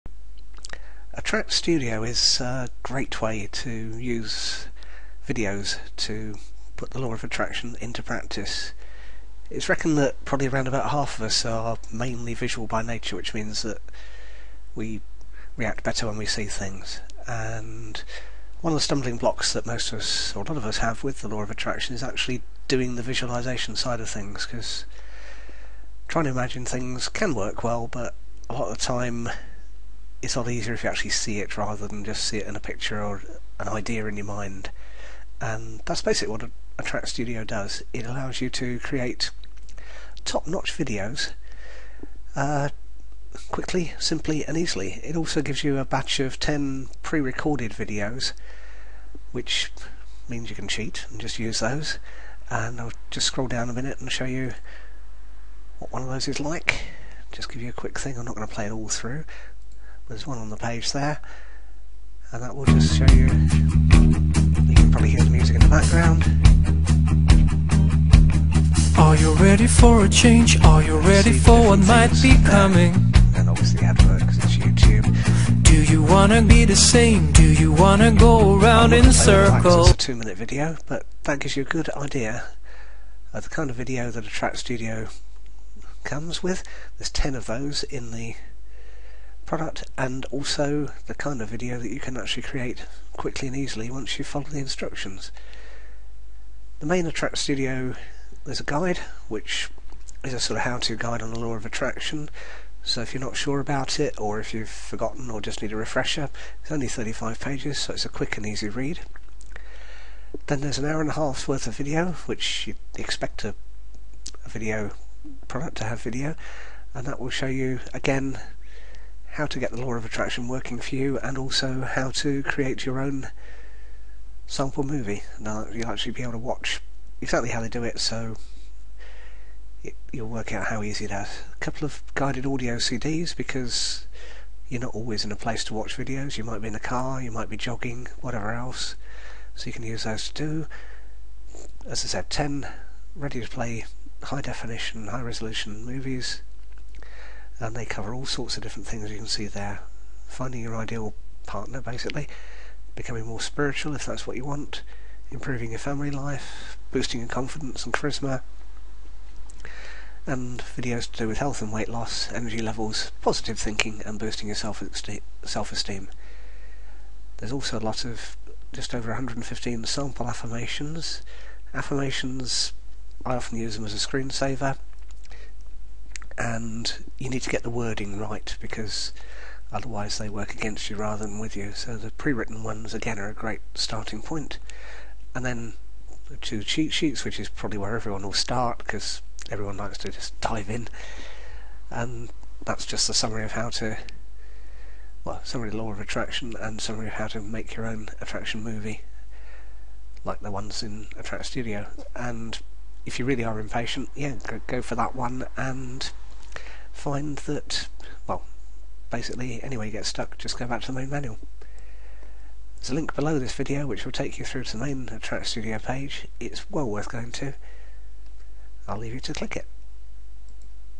Attract Studio Review
Attract Studio is a comprehensive course that shows you how to make your own personalized law of attraction movies.

It's generally thought that about half of us are completely visual by nature and a large number of the rest of the population have visual as there second method of learning. Attract Studio caters for visual people by providing a set of ten ready made law of attraction videos plus complete instructions on how to make your own law of attraction movies.

Attract Studio is a great way to help put the law of attraction to work, manifesting health, wealth, happiness, self confidence and much more into your life.

The Attract Studio package is detailed, easy to follow and comes with a 60 day money back guarantee.